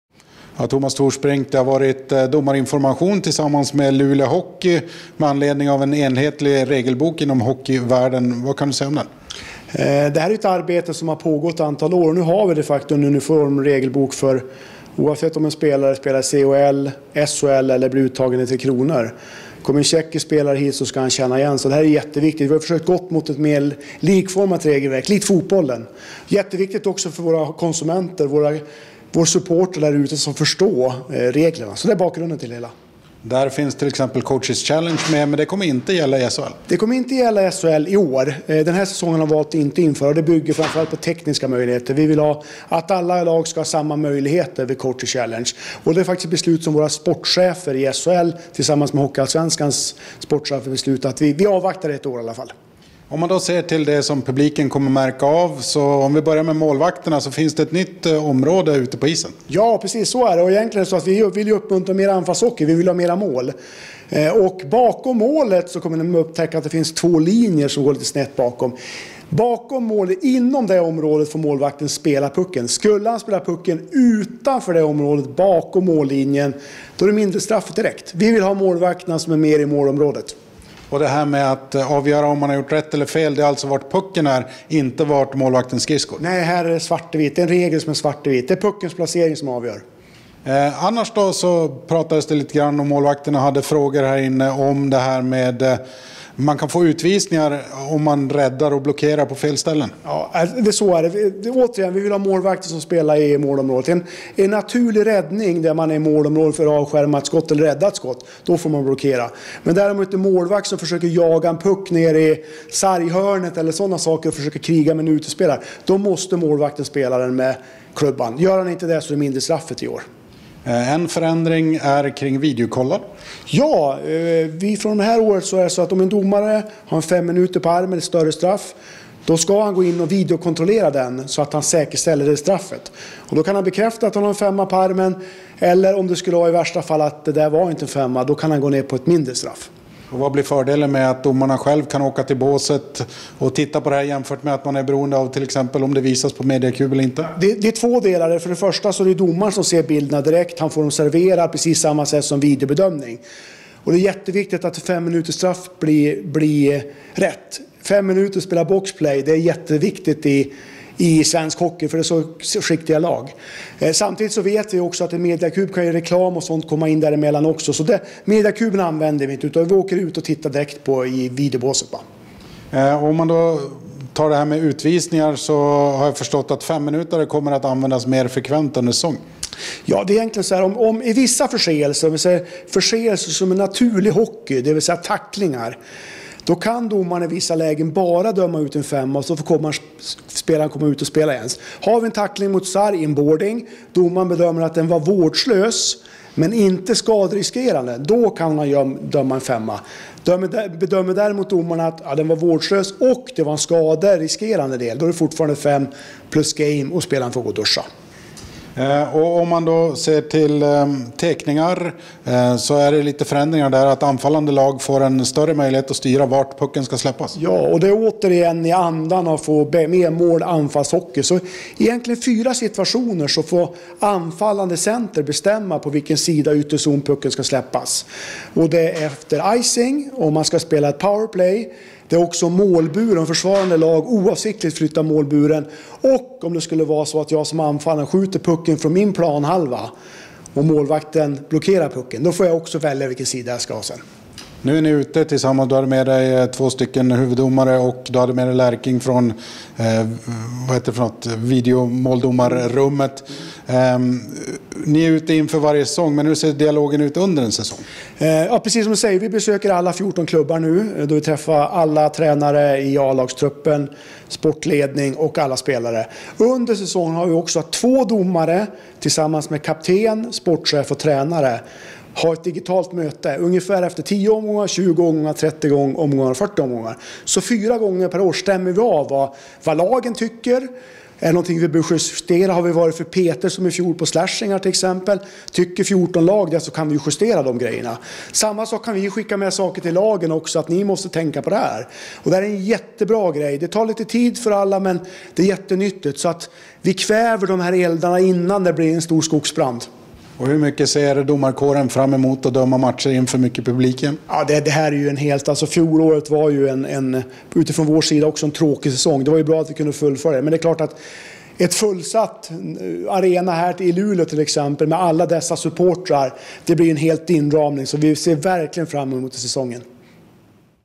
"Gått mot ett mer likformat regelverk"
SHL: s domarchef Tomas Thorsbrink berättar om den nya enhetliga regelboken samt en del regelnyheter.